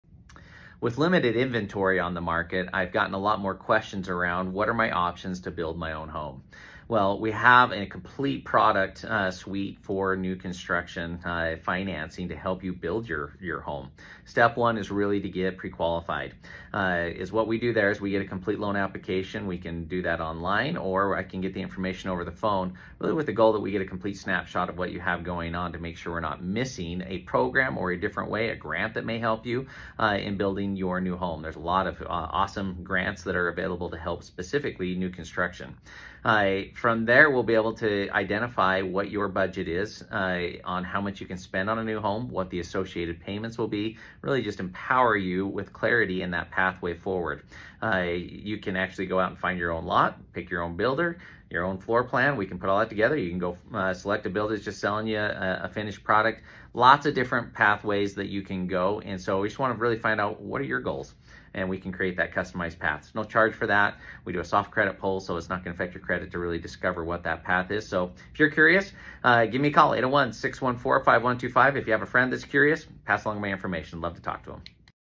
Your Financial Pathway To Building a New Home
Curious about building your next home, here are some key tips on how to find out what that pathway and options look like for you. If you have a friend or family member with questions please share this with them as well.